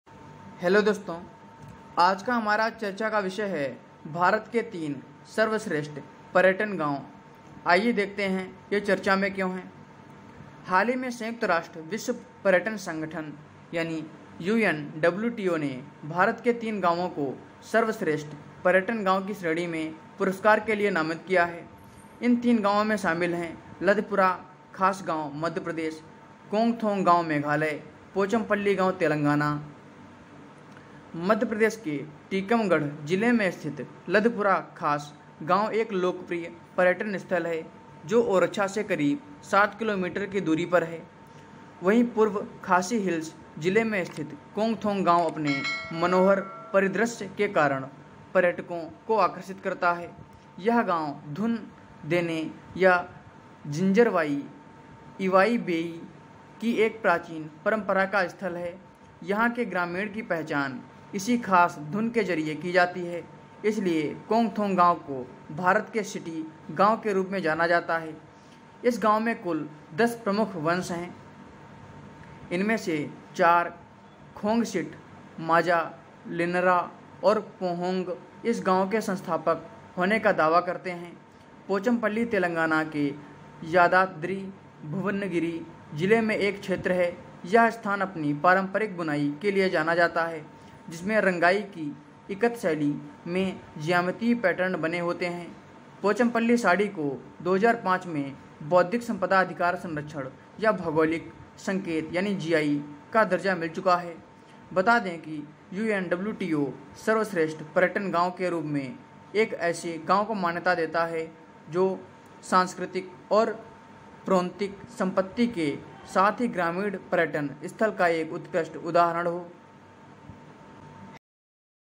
3 Indian Destinations Are Among Entries For UN's 'Best Tourism Village
3 Indian Destinations Are Among Entries For UN's 'Best Tourism Village